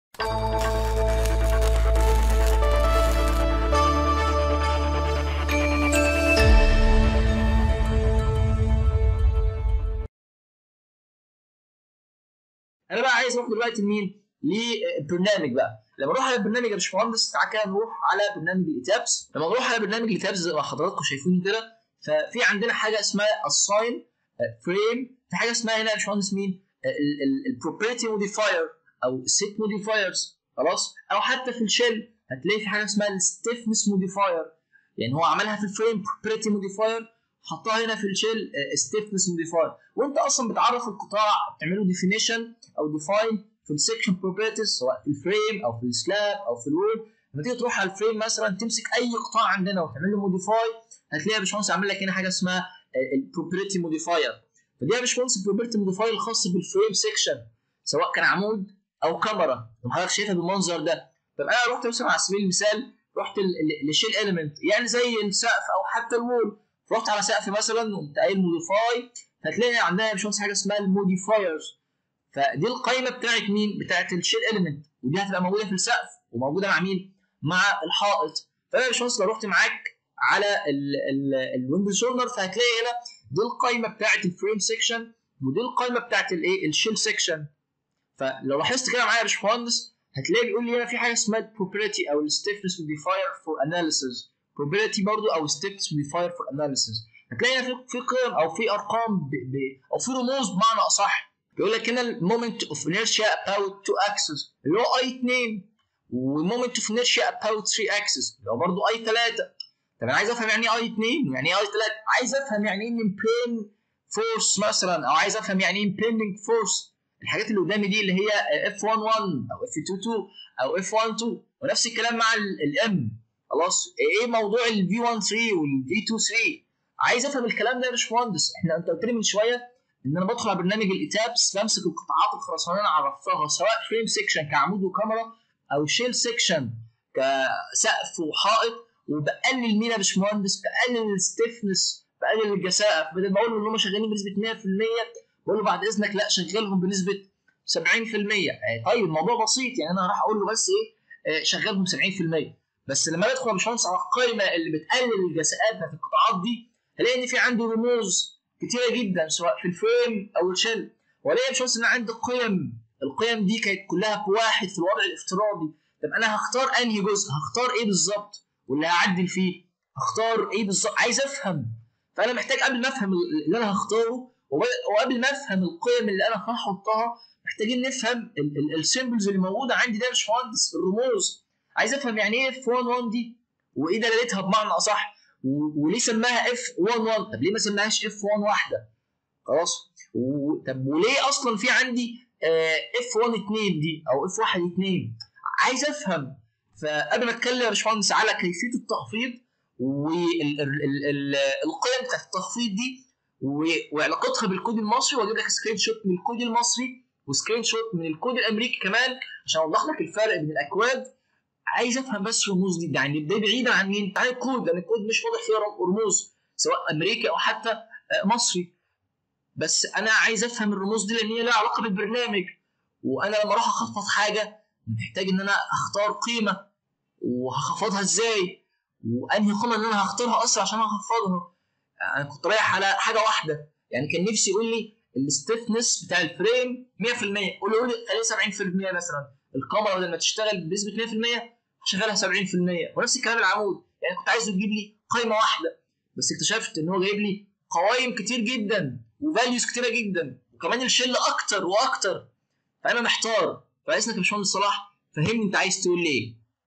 Set Modifiers for Structural Elements Part 1
تخفيض الجساءات للعناصر الانشائية المعرضة للاحمال الجانبية

م/ صلاح سالم
معيد بقسم الهندسة الإنشائية - جامعة المنصورة

لو عايز تعرف اكتر عن دبلومة التصميم الإنشائى
شوف اللينك دا: